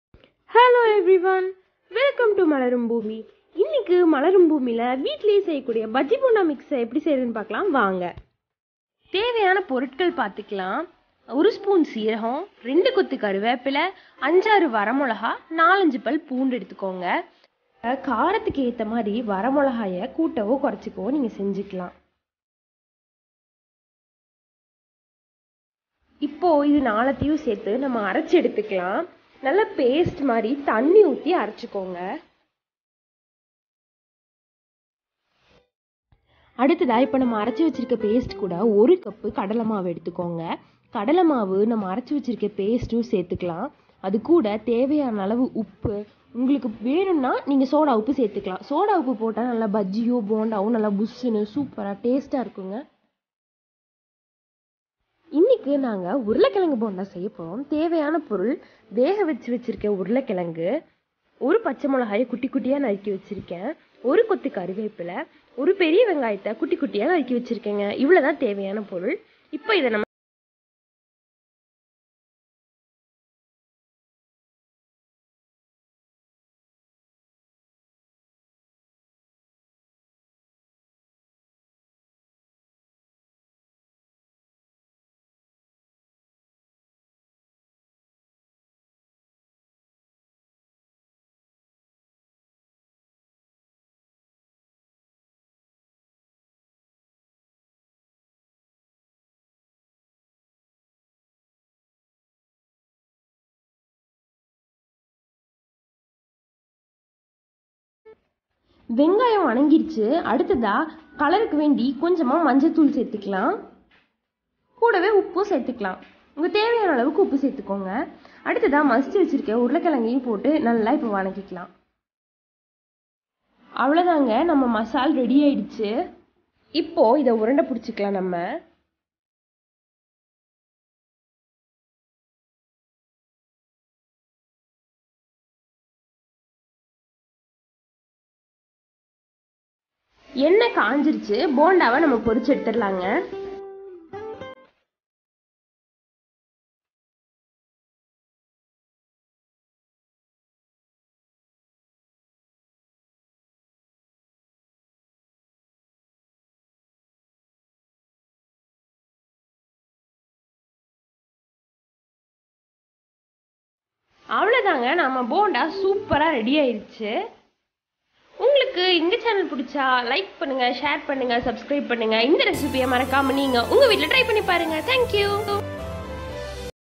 easy TRADITIONAL Bonda mix taste guranteed malarum bhoomi
learn to make home made bajji bonda mix so easy and so tasty
I tried this one after that never bought bajji bonda mix from stores...